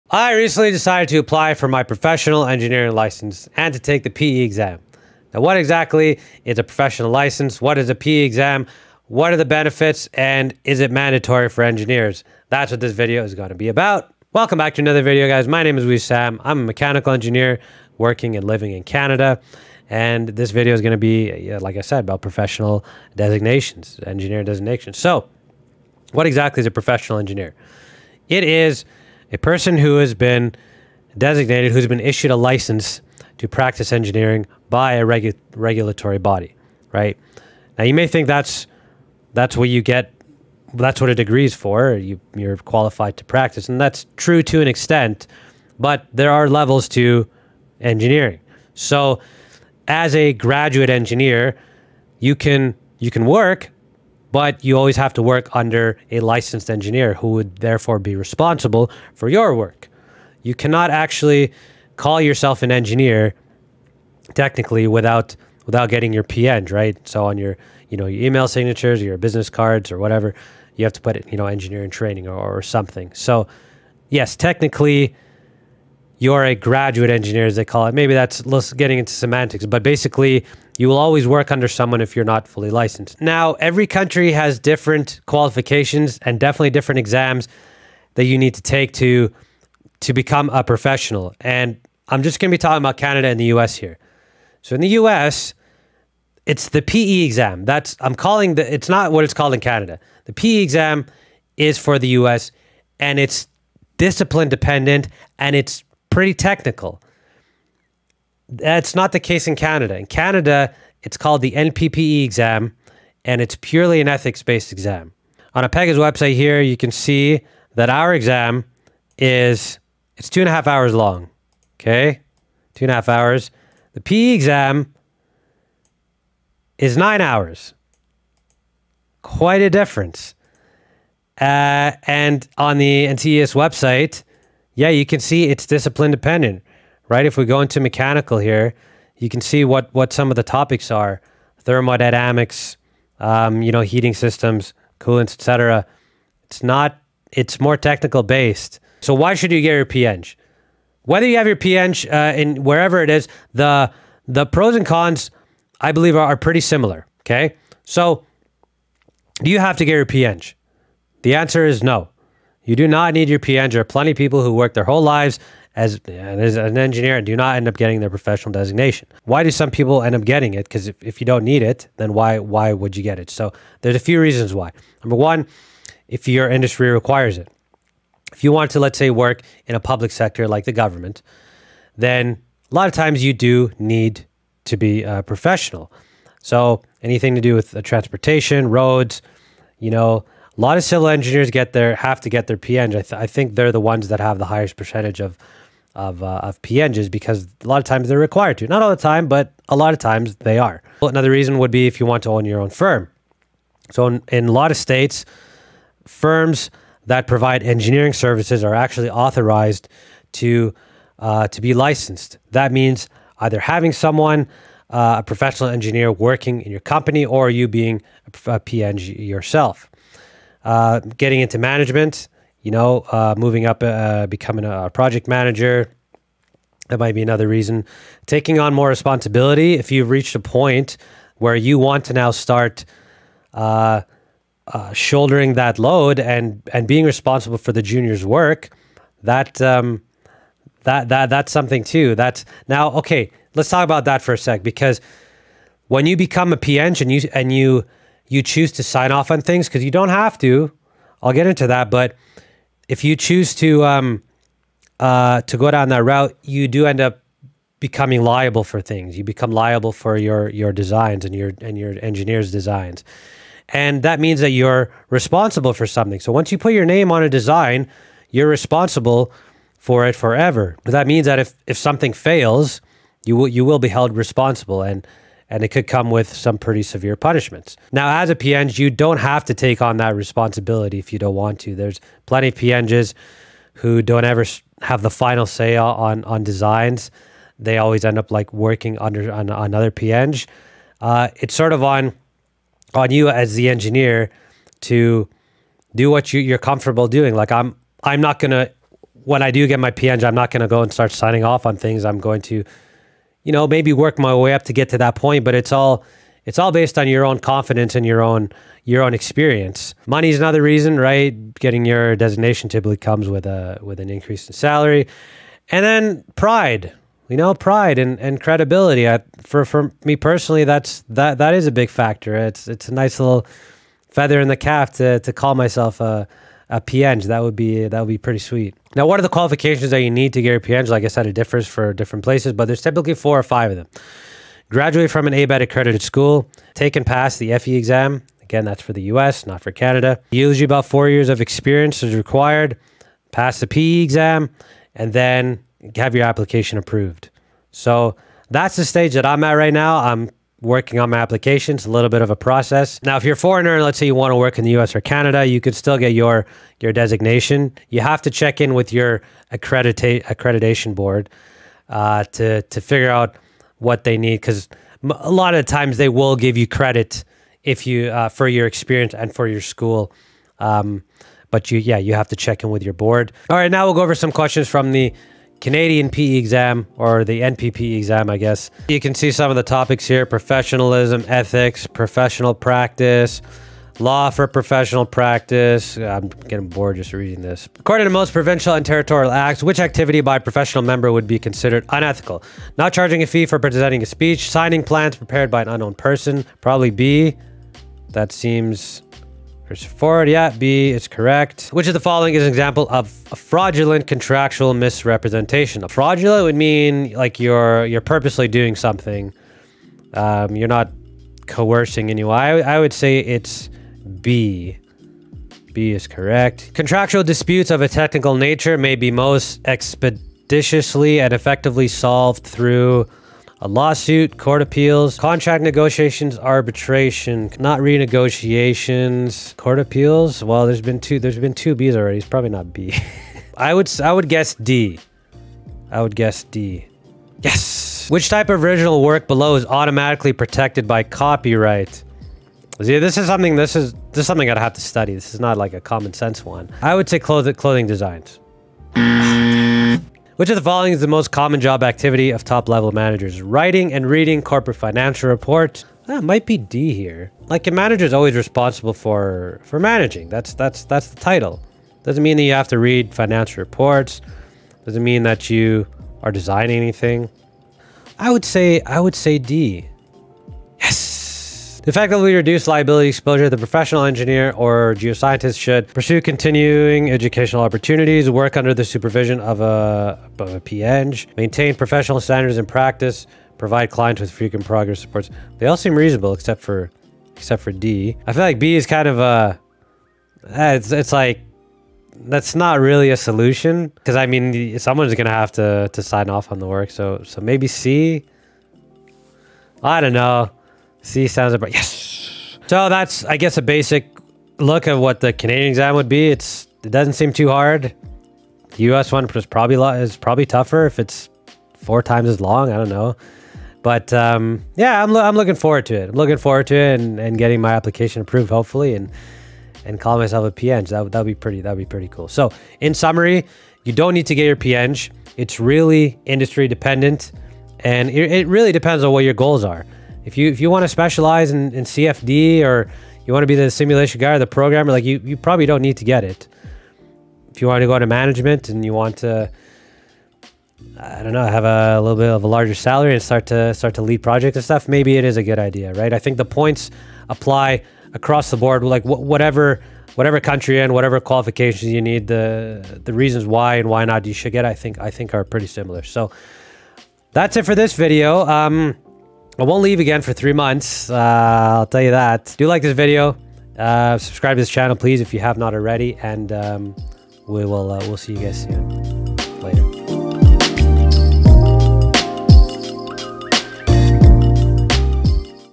How To Study For The Engineering LICENSE EXAM
Mistake I made at the beginning: I'm NOT taking the PE exam, as I live in Canada. I'm taking the NPPE exam which is quite different. Either way I think the reasons for and against getting your professional designation are similar for everyone. We'll see how mine goes!